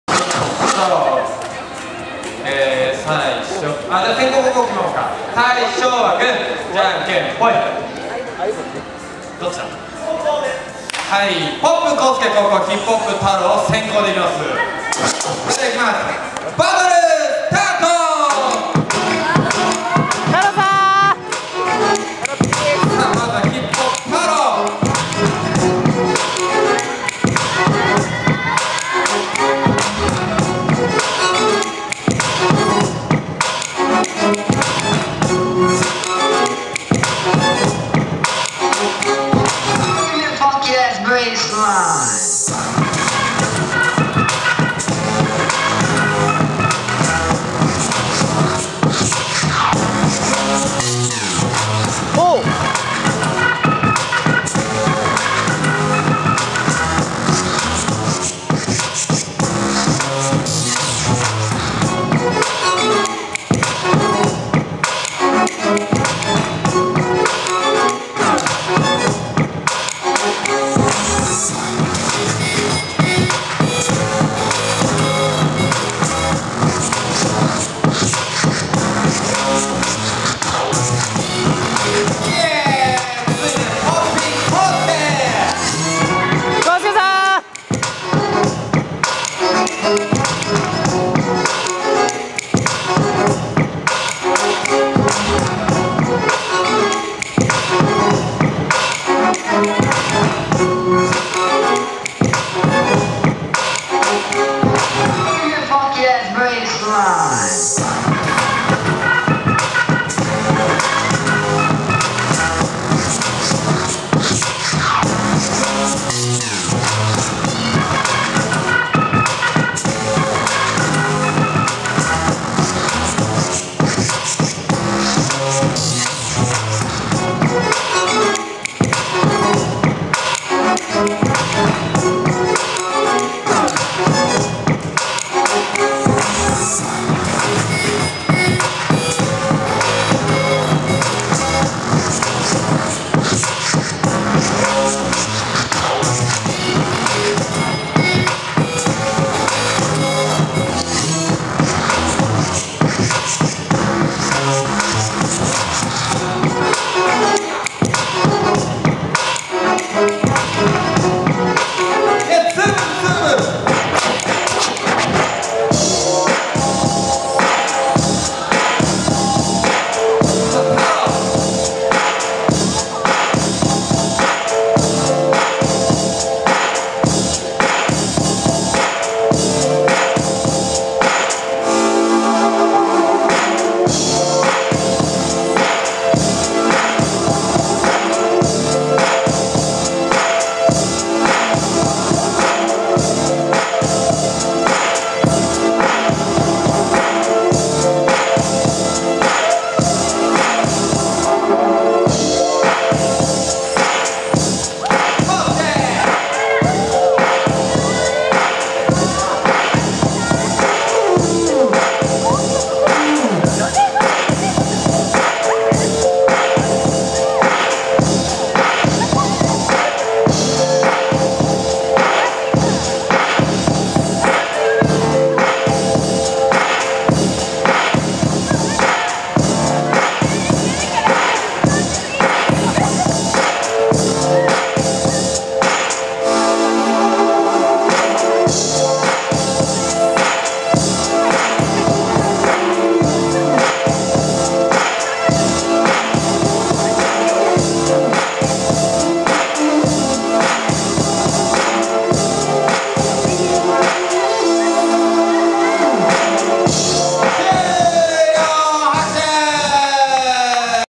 TENDO FES. [KIZUNA] DANCE BATTLE FREE STYLE BEST 8
山形県天童市にて行われたBATTLEの模様

TARO  (HIPHOP) VS KO-SUKE  (POP)

WINNER. TARO !